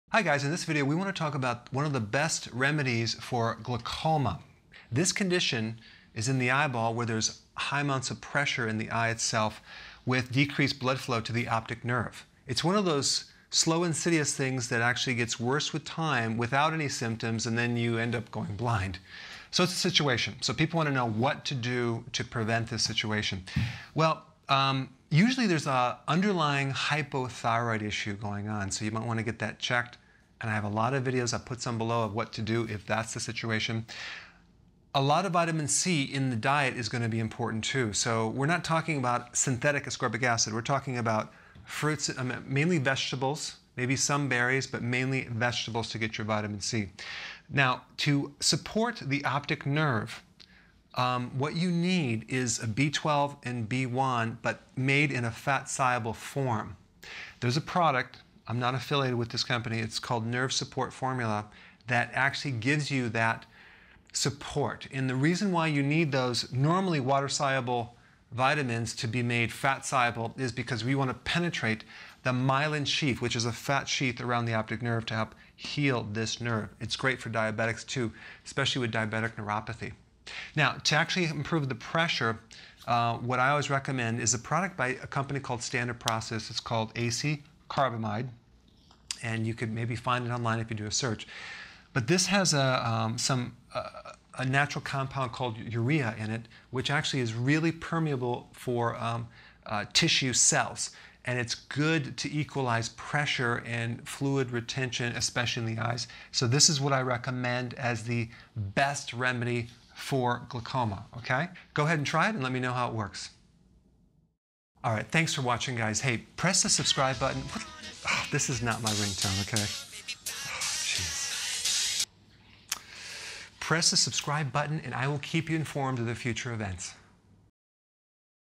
How To Improve Glaucoma? – Dr.Berg On Glaucoma Treatment
Dr. Berg talks about glaucoma and what remedy to use to prevent it. The condition can negatively affect the optic nerve due to pressure internally. I recommend using a Standard Process product called AC Carbamide - do a search on this or find a local healthcare practitioner who sells it.

If there is nerve damage, I recommend:
Nerve Support Formula (do a search on it - these are fat-soluble B vitamins)..